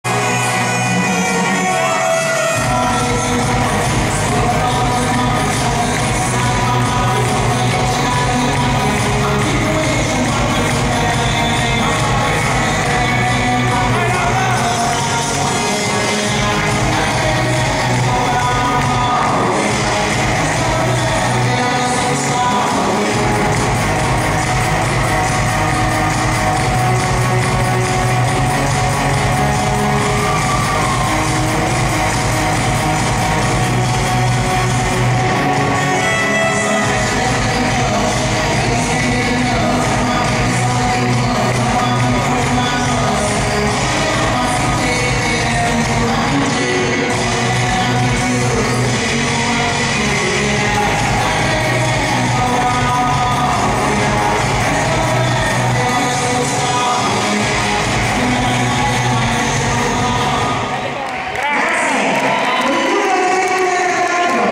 2^ Tappa Di Coppa Italia 2015 - Disco Dance - LAURA SIMONINI
Classe AS
Categoria ''Over 16 F''
Regione Liguria
FIDS
1^ Classificata